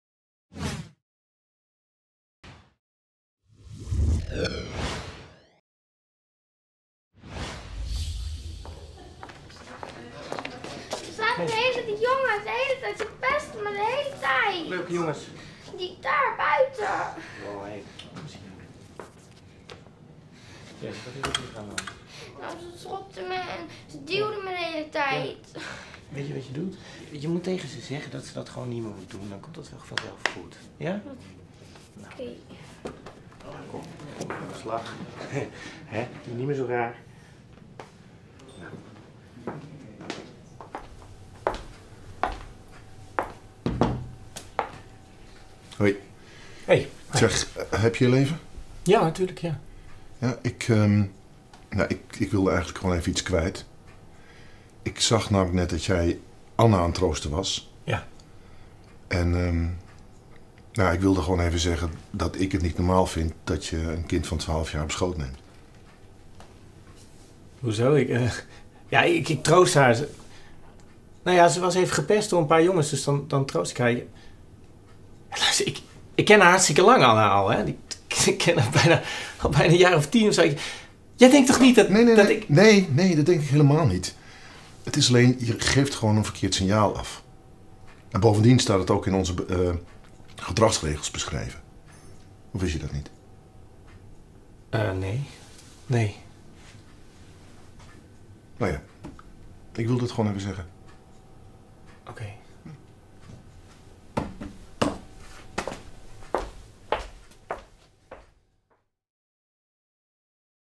Situatieschets: Kind troosten
In deze situatieschets spelen acteurs een situatie na die je bij vrijwilligersorganisaties tegen kunt komen.

Discussiepunten:
• Past deze vorm van troosten bij een 13-jarige?
• Bij welke leeftijd kan dit nog wel en bij welke niet meer?
• Wat als dit een jongetje was geweest? Wat als de vrijwilliger een vrouw was geweest?
• Wat zou het vervolg van deze situatie kunnen zijn?
• Vind je dat een vrijwilliger de verantwoordelijkheid heeft om een andere vrijwilliger hierop aan te spreken?
• Wat zou het makkelijker kunnen maken voor een vrijwilliger om een collega aan te spreken?
• Is het nodig om dit voorval aan ouders te vertellen?
• Wie zou kunnen helpen?
• Hoe kan een vrijwilliger weten wat de normen en waarden in een organisatie zijn?

Er bestaat ook een uitgebreide Toolkit die gratis te downloaden is op de